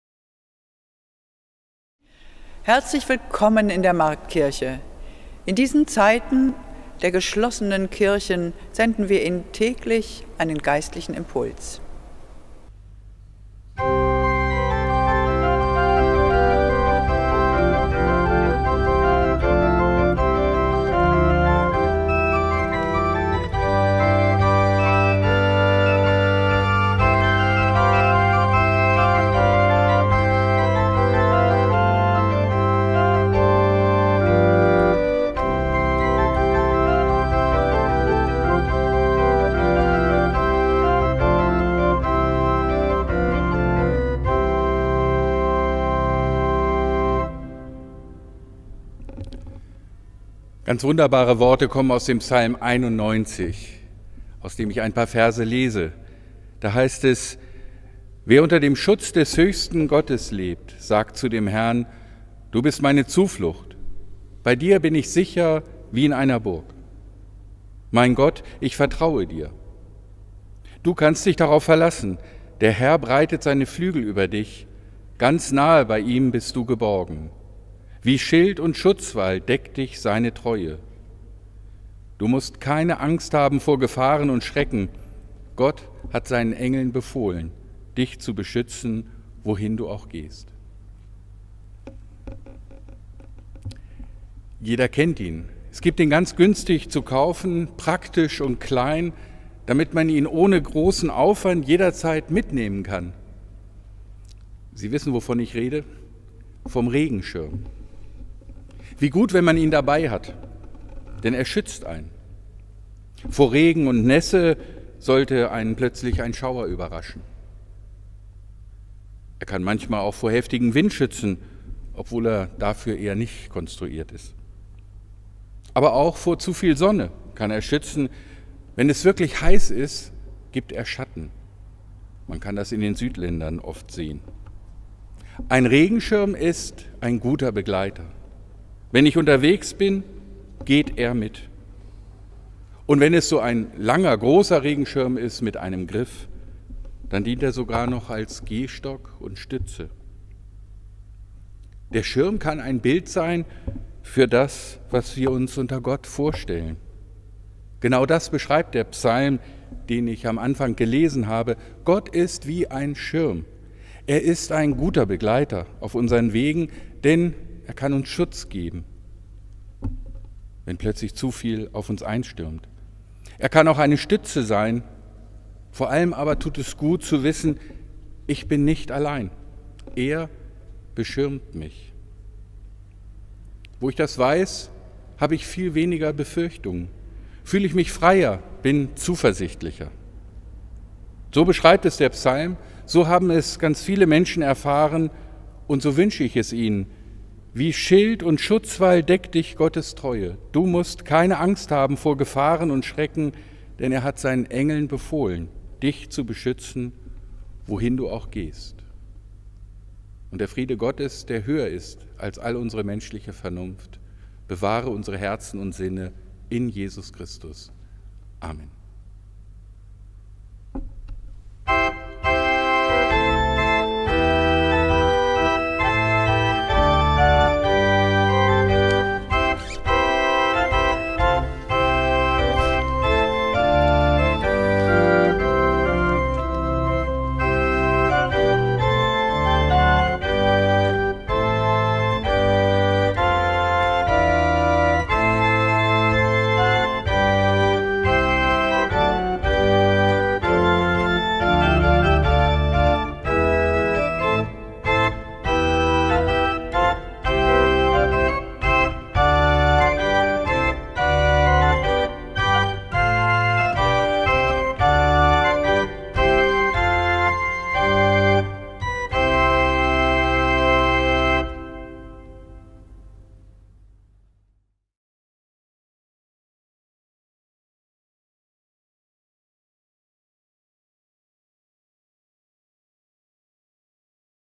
Geistlicher Impuls am 24.03.2020
In Zeiten geschlossener Marktkirche grüßen wir Sie täglich mit einem 'Geistlichen Impuls' aus der Marktkirche Hannover.